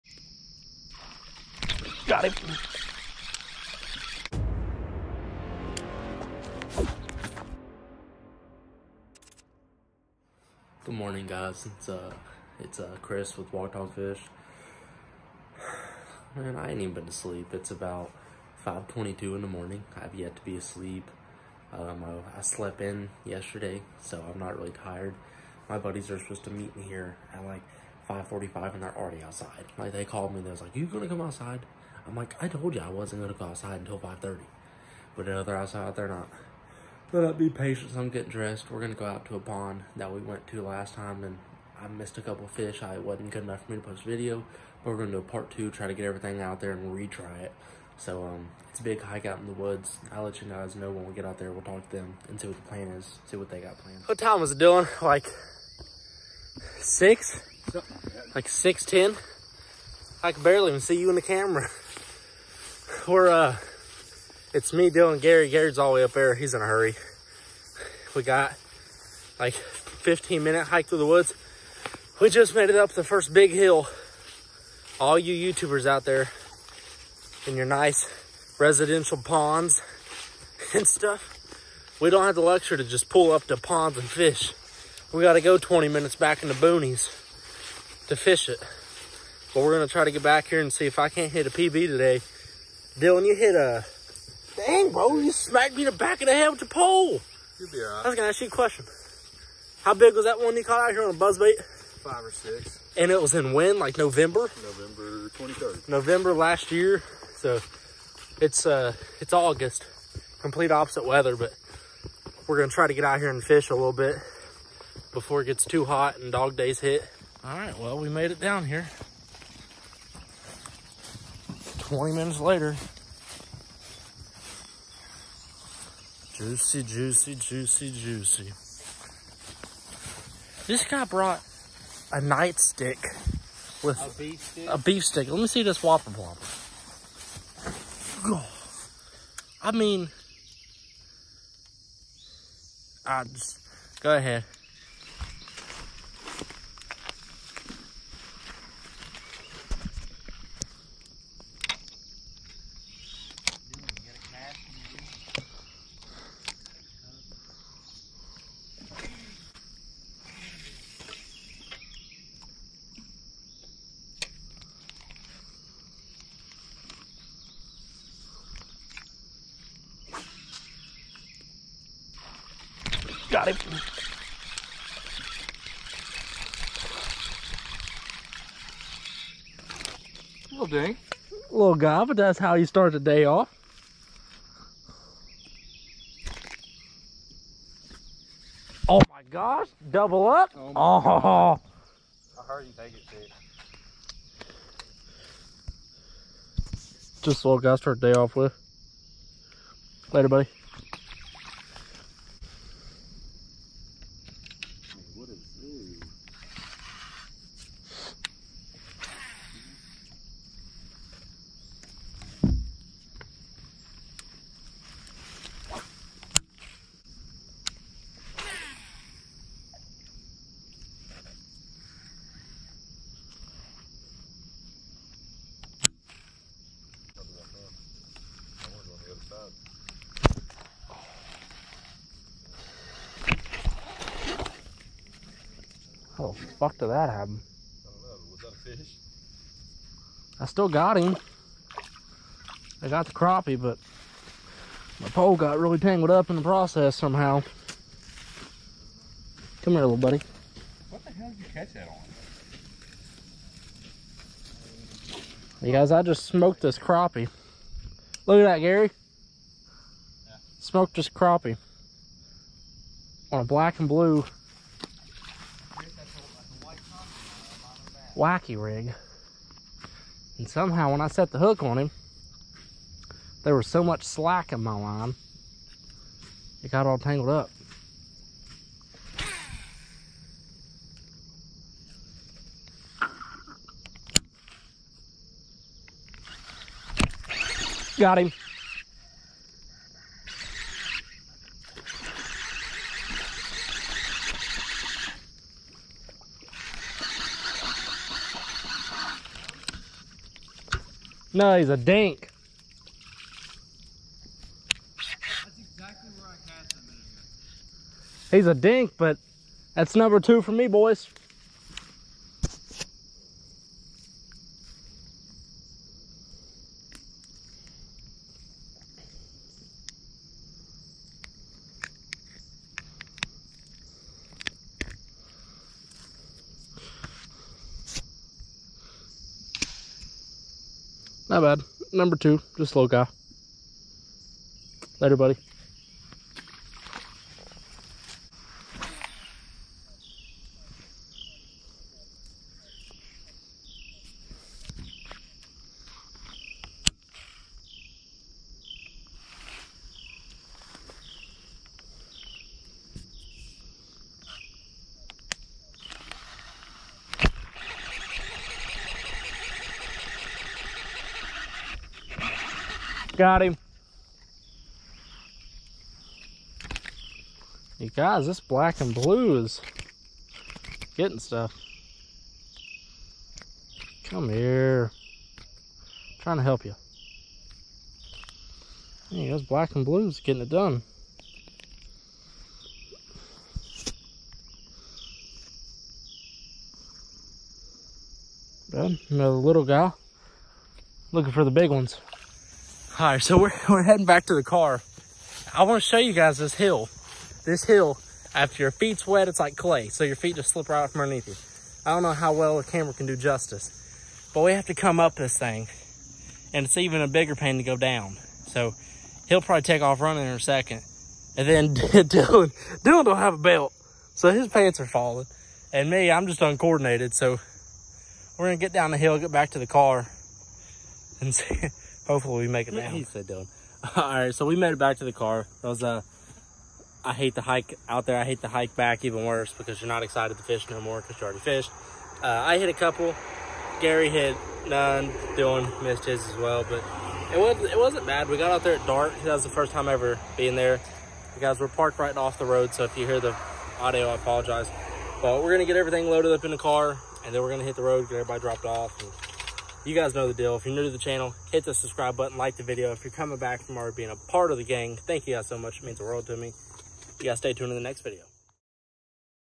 Deserted Pond! ABSOLUTELY LOADED WITH FISH!
Finally hit some largemouth bass at this airport pond! Hit 1 on the top water googan frog and the others on black and blue soft plastics